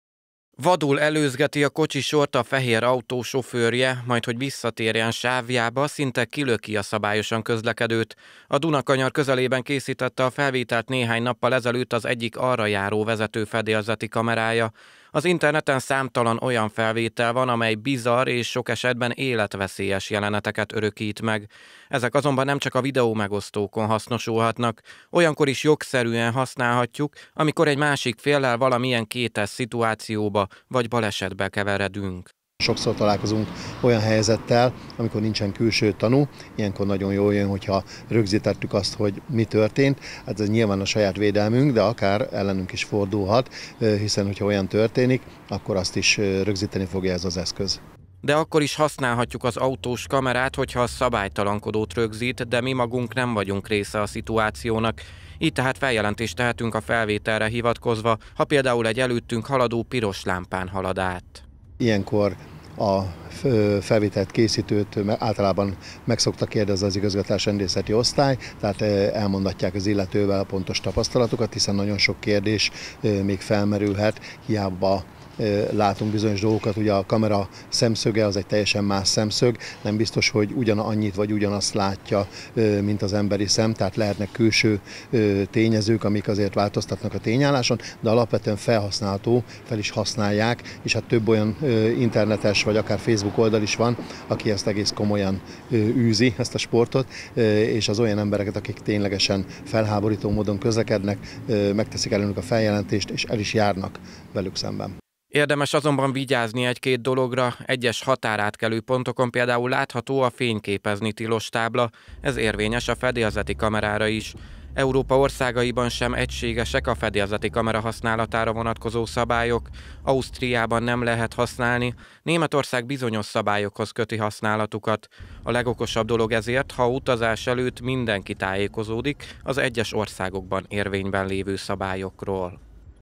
Felhasználható a fedélzeti kamera
Továbbra sincs egységes, jogi szabályozás a fedélzeti kamera használatát illetően. Olyan jogszabály sincs azonban Magyarországon, amely tiltaná a fedélzeti kamera használatát. A rendőrség most megerősítette, az eszköz jogszerűen használható és akár bizonyító erejű is lehet a kétes szituációk vagy baleset esetén. Külföldre utazás előtt ugyanakkor érdemes utánanézni a szabályoknak: egyes országok ugyanis, mint például Ausztria, tiltják az autós kamera használatát.